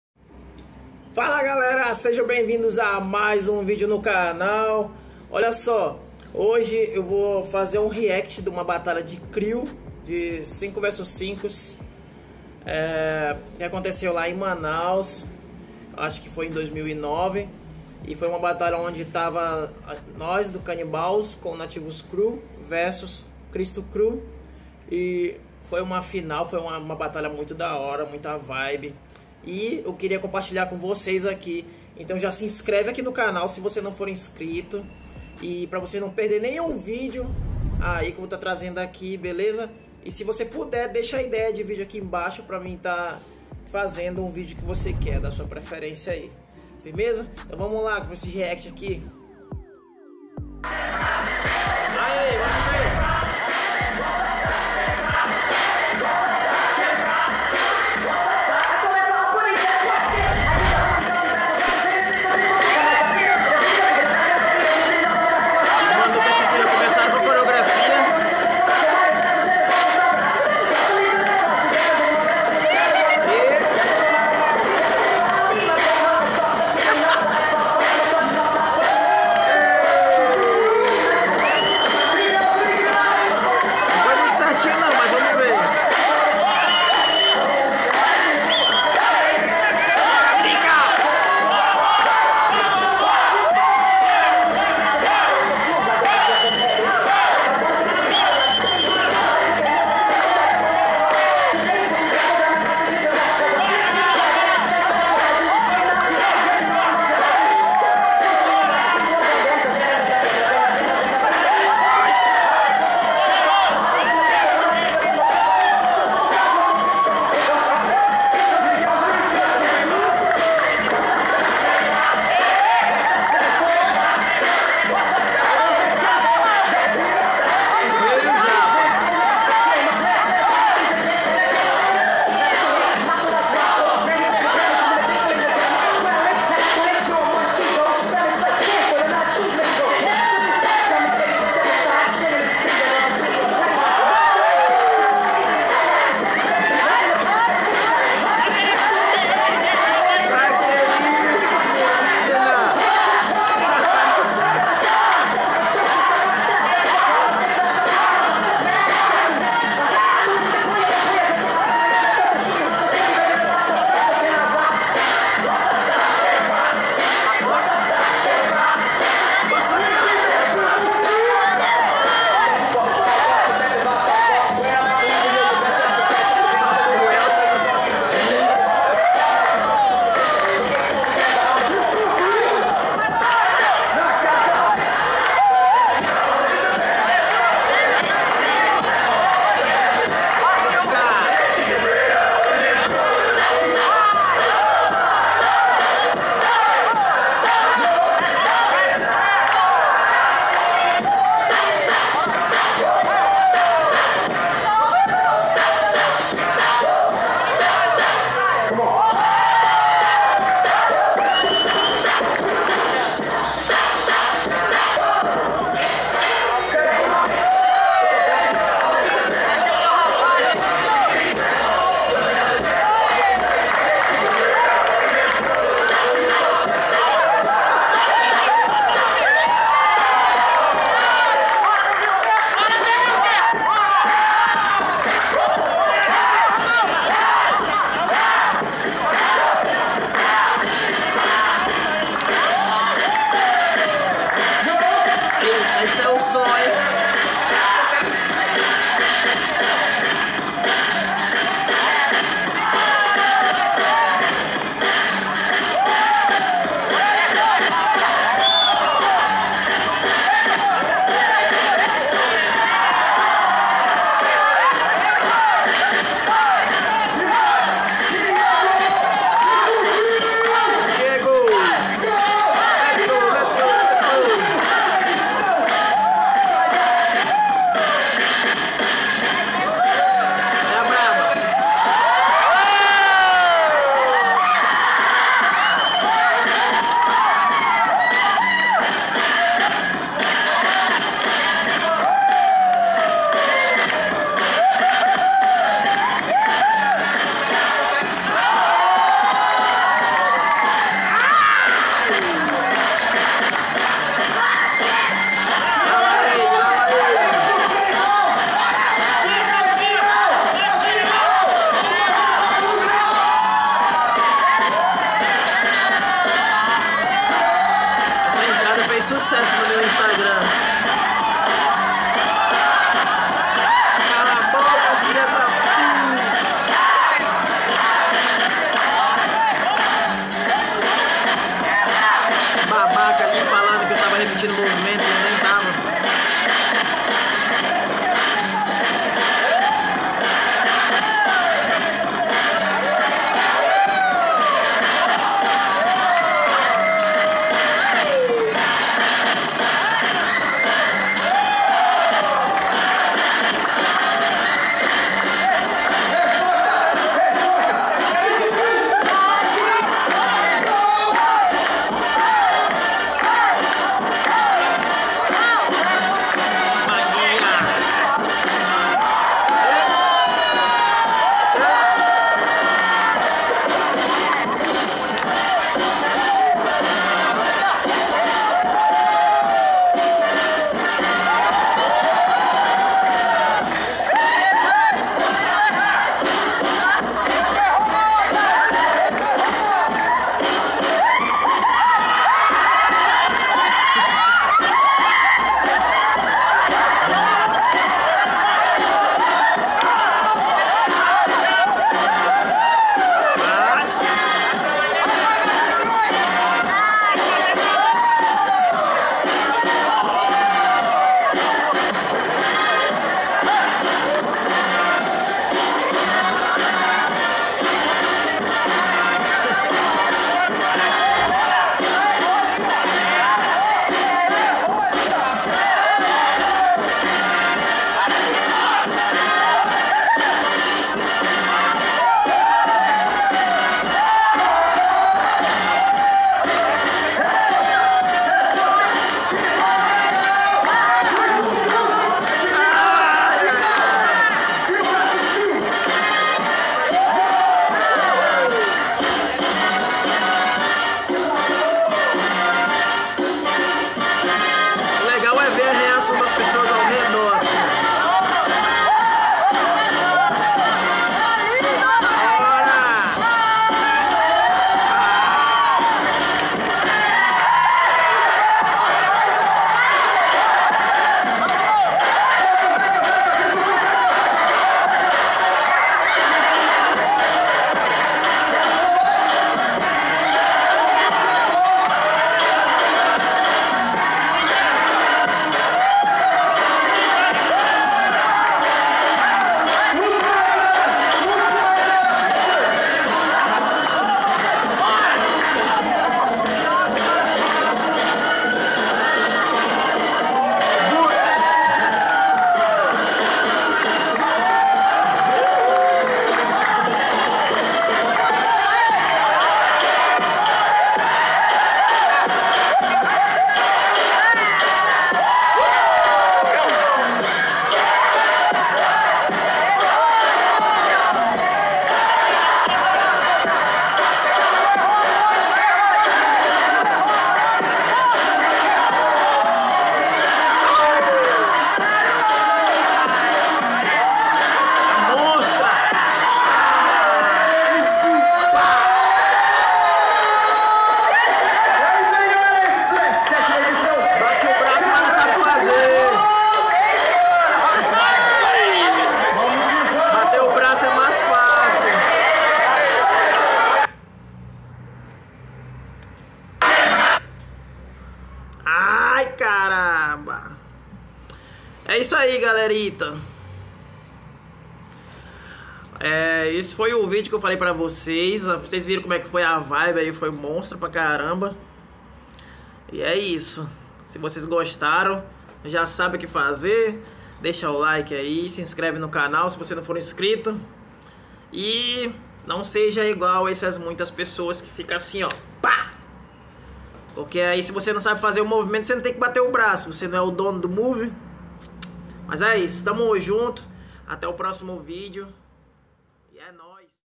REACT BATTLE - CRISTO CREW VS NATIVOS & R KANNYBAL'S / TRIBO X TRIBO MANAUS
Fala galera neste vídeo de hoje é um React de uma batalha TRIBOX tribo organizado pelo grupo nativos crew em Manaus e eu trouxe pra vocês porque achei que seria interessante.

Página Team Floor Kings